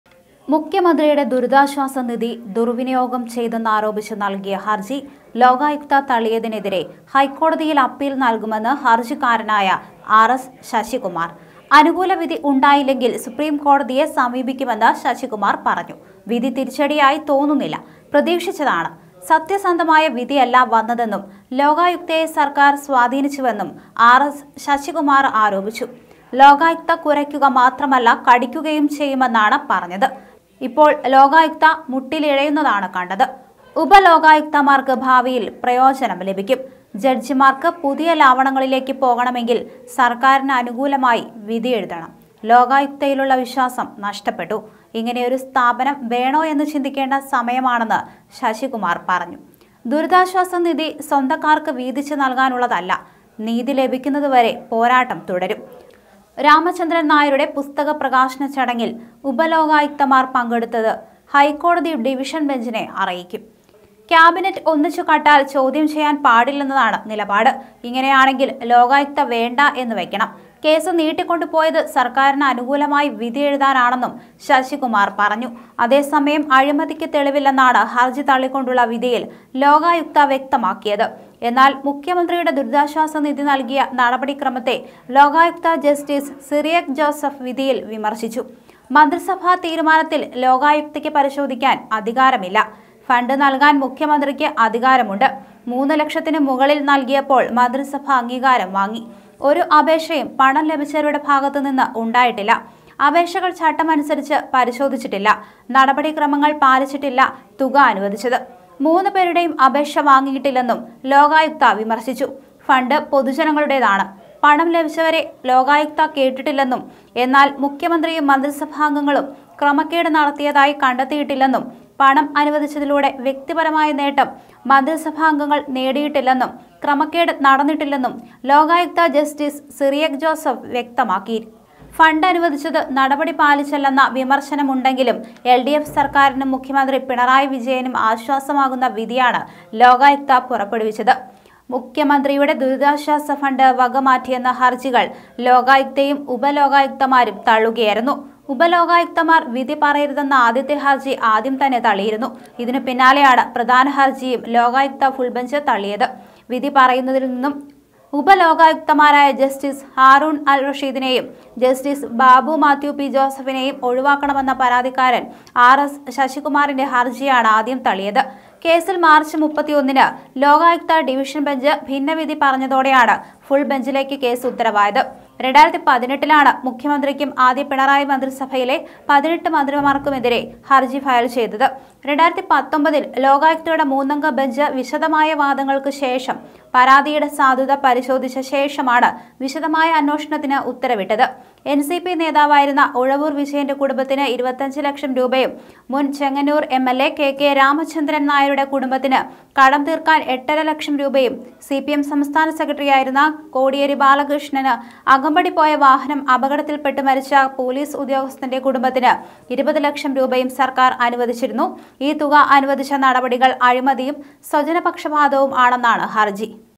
വിധിക്കെതിരെ ഹൈക്കോടതിയില്‍ അപ്പീല്‍ നല്‍കുമെന്ന് ഹര്‍ജിക്കാരനായ ആര്‍ എസ് ശശികുമാര്‍ I lokayukta
ലോകായുക്ത മുട്ടിലിഴയുന്നു; സത്യസന്ധമായ വിധിയല്ല വന്നത്; ലോകായുക്തയെ സർക്കാർ സ്വാധീനിച്ചു; ഇങ്ങനെയൊരു സ്ഥാപനം വേണമോയെന്ന് ചിന്തിക്കണം; വിധിക്കെതിരെ ഹൈക്കോടതിയിൽ അപ്പീൽ നൽകുമെന്ന് ഹർജിക്കാരനായ ആർ എസ് ശശികുമാർ